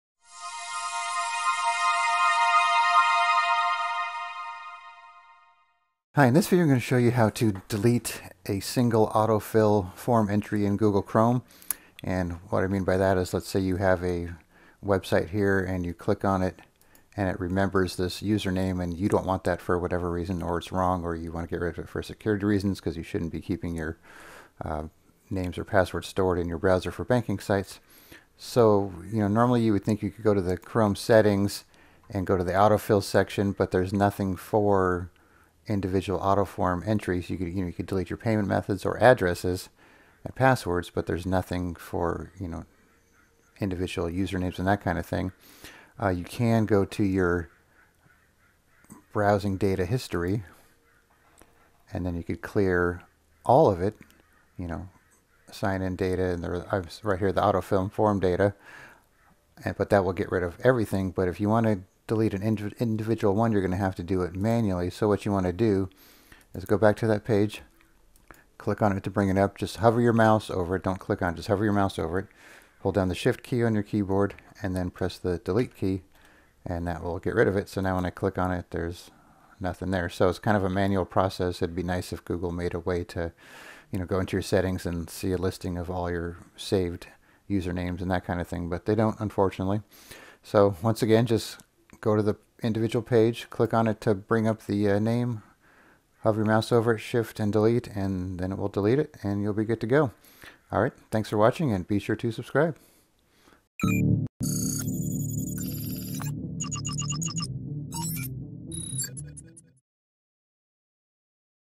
How to Delete a Single Google Chrome Autofill Form Entry
If you have any websites that have saved information such as usernames in the online form and you want to clear them out, then it's very easy to do but unfortunately is a manual process to do so. Chrome doesn't have an option in the settings that will allow you to see your saved form data in your browser unless its addresses or credit cards etc. so you will need to do it this way.